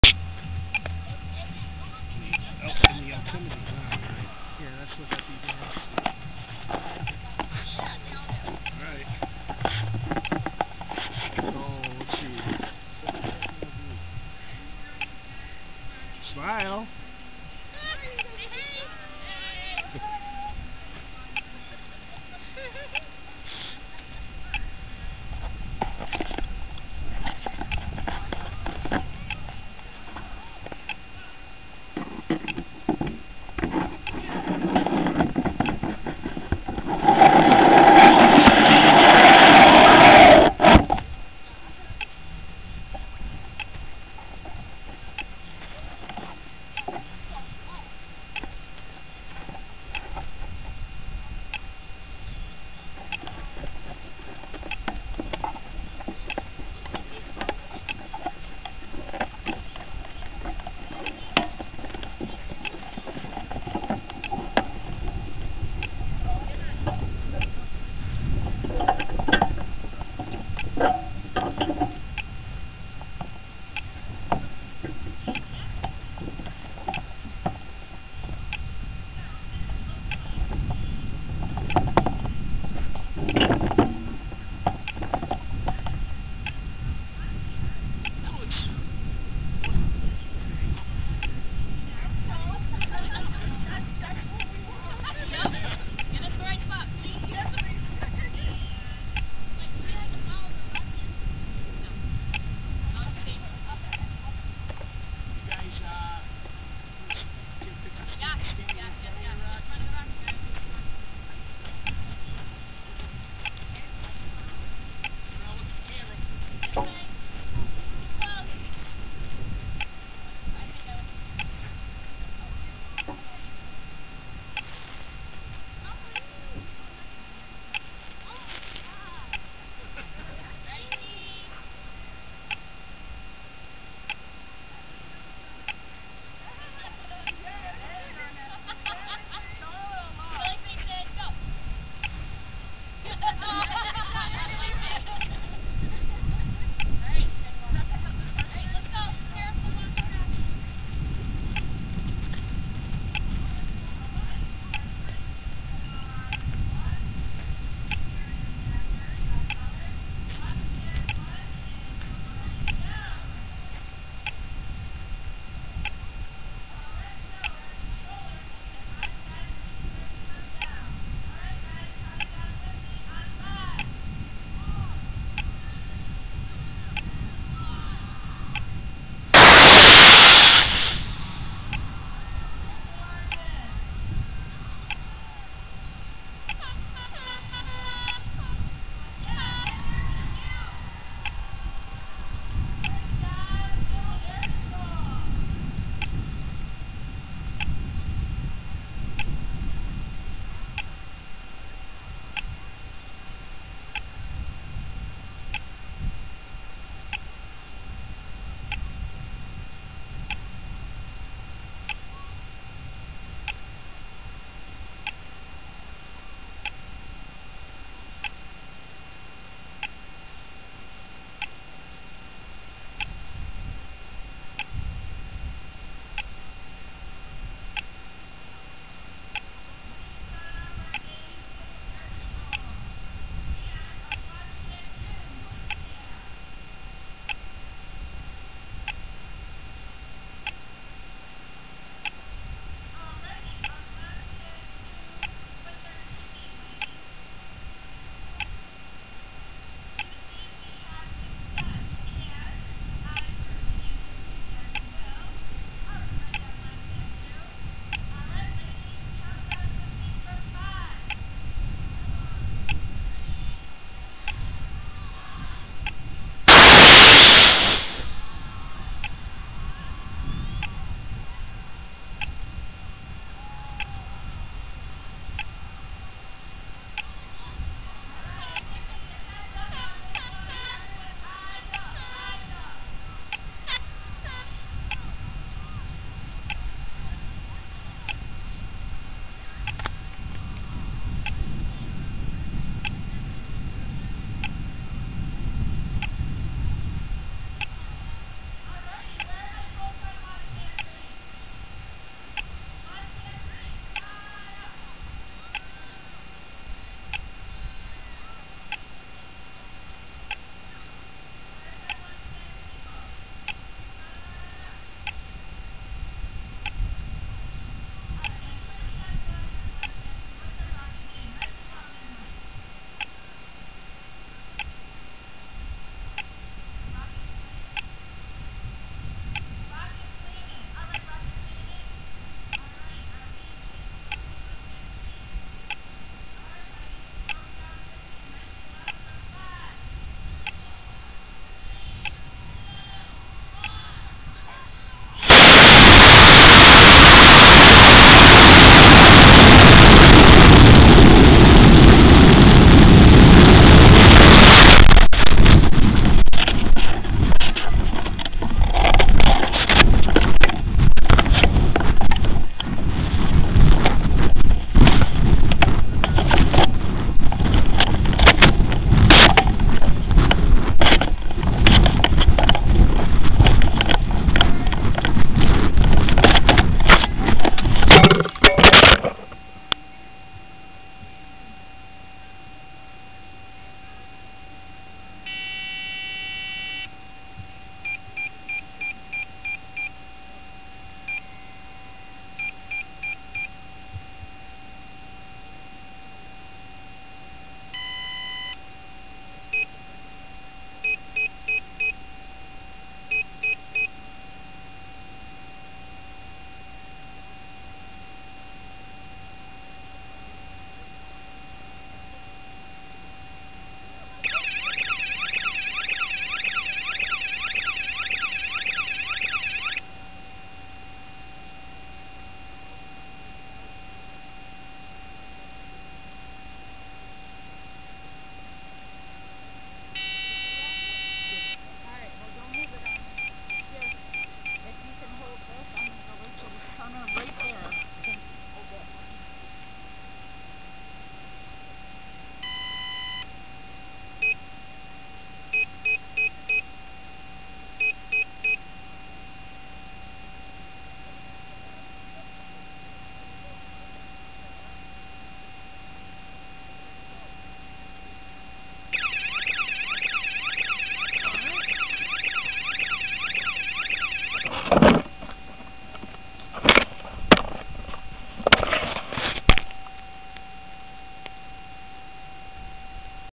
2018 CNY Rocket Team Challenge Team 938
This video was recorded on a rocket launched by Team 938.